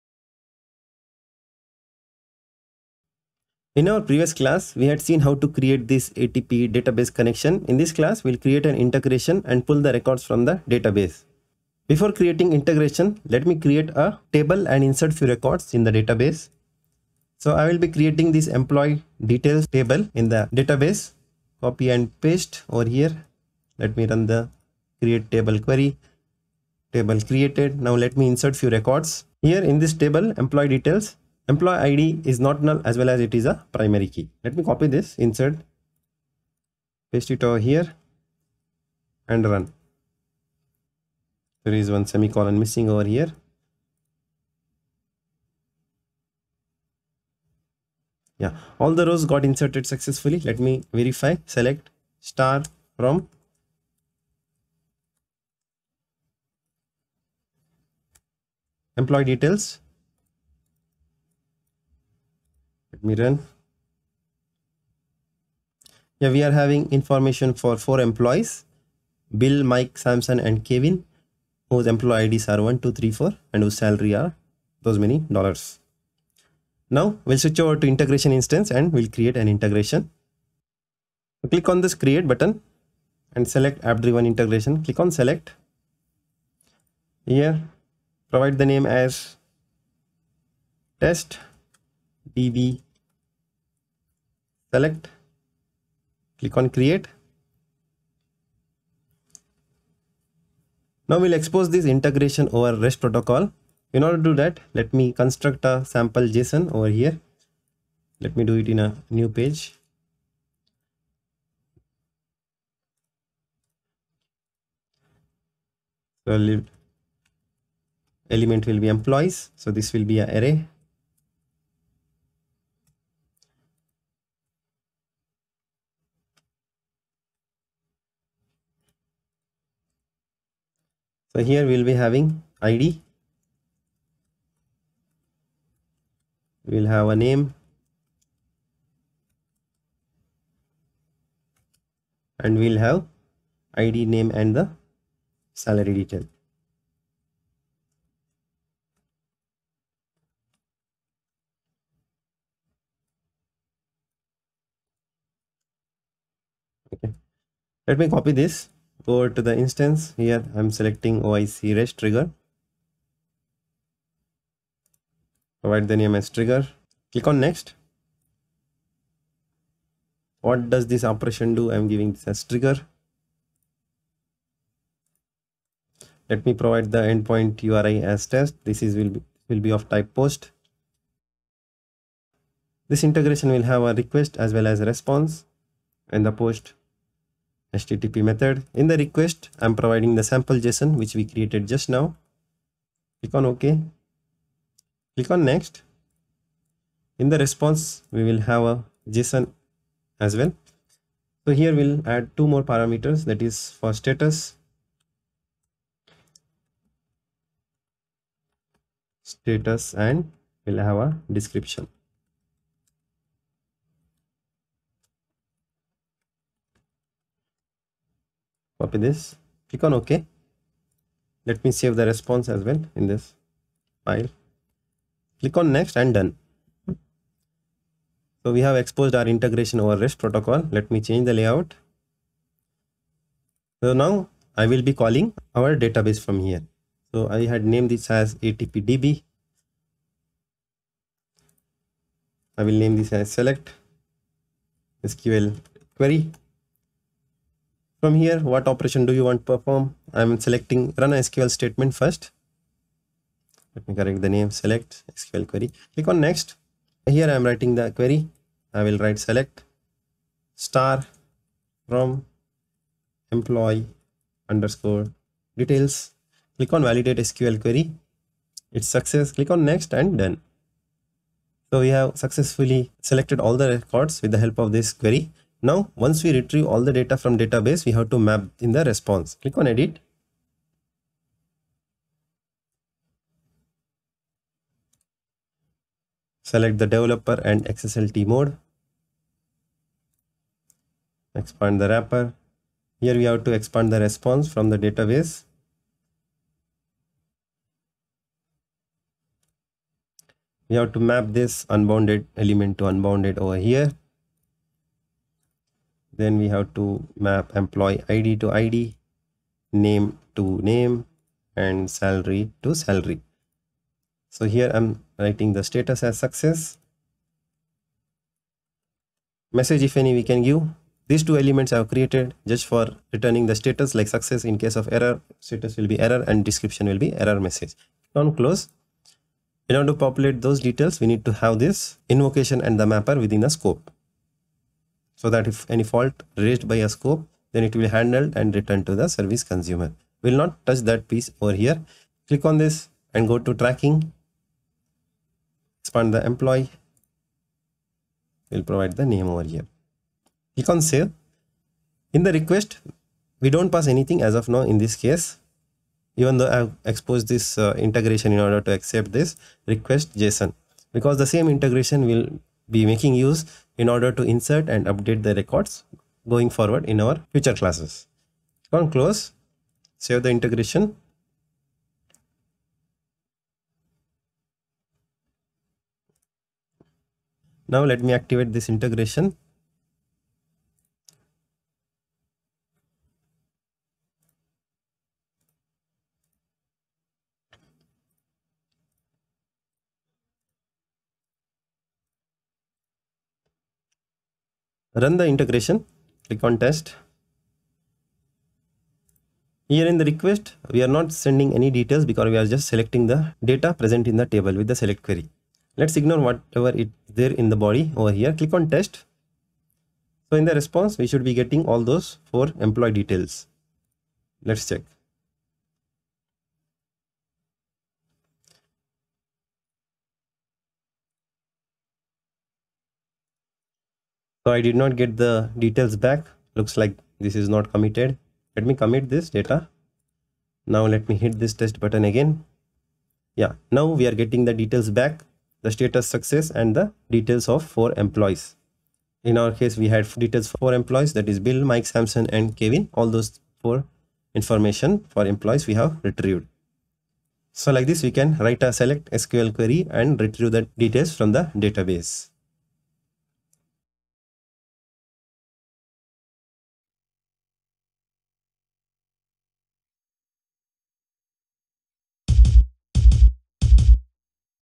How to fetch records from Oracle ATP database with SELECT SQL query? Oracle Integration (OIC)
With this video we will learn How to fetch records from Oracle ATP database with SELECT SQL query? Oracle Integration (OIC) | Run a SQL statement | SELECT

0:00:03 - What you'll learn with this tutorial
0:00:11 - Create table in Oracle ATP database and insert few records into table
0:01:20 - Create App driven integration to fetch records from ATP database
0:03:53 - Invoke Oracle ATP database connection with Run a SQL statement operation (SELECT SQL query)
0:06:35 - Testing and Validation